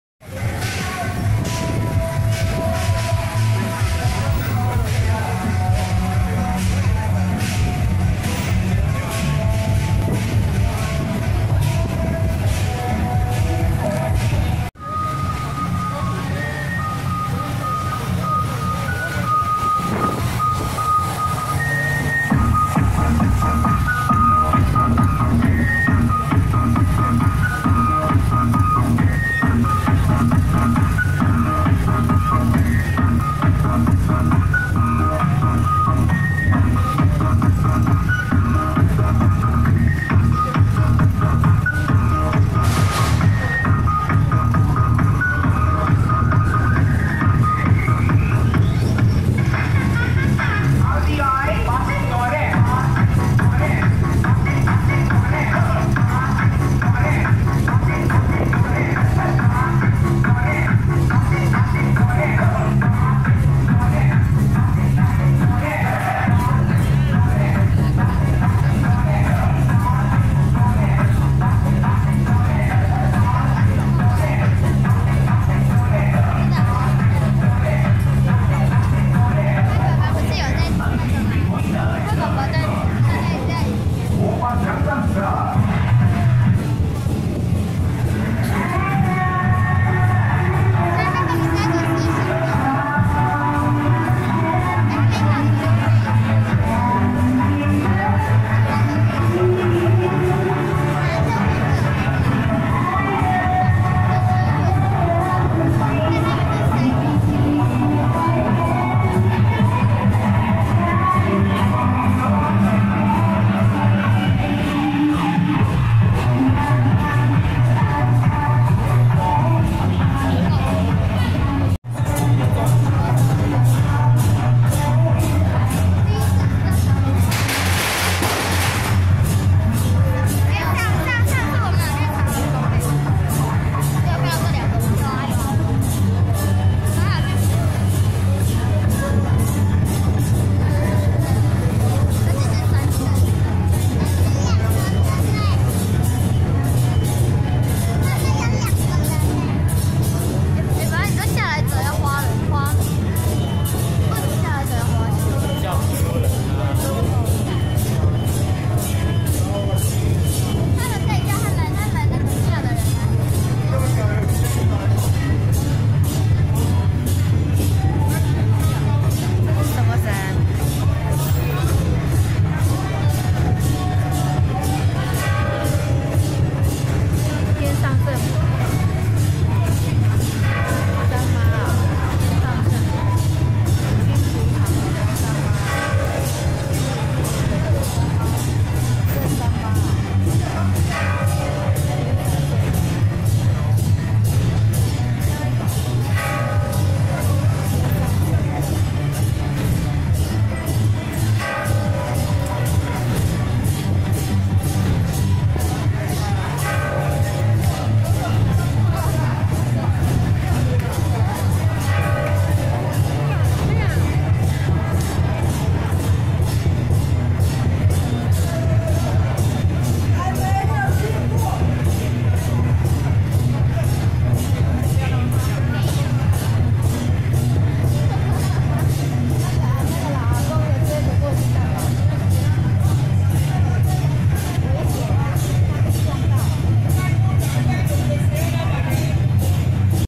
107新竹聖安宮開漳聖王民俗文化祭遶境【新竹長和宮 慶贊 新竹聖安宮】
(0:00)新竹長和宮-頭旗西燈車
(0:17)竹塹電音太子團
(2:13)新竹長和宮-千順將軍
(2:45)新竹長和宮-天上聖母正三媽 聖駕